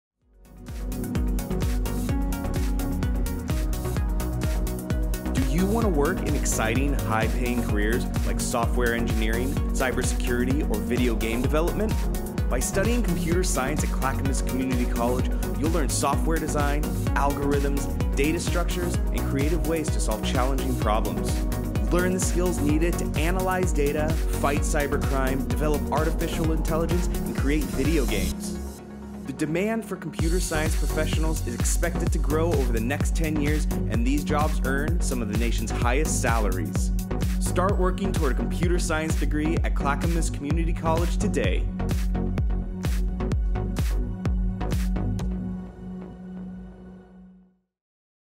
Learn Computer Science at CCC!
Earn your Associate of Science Transfer Degree in Computer Science at Clackamas Community College.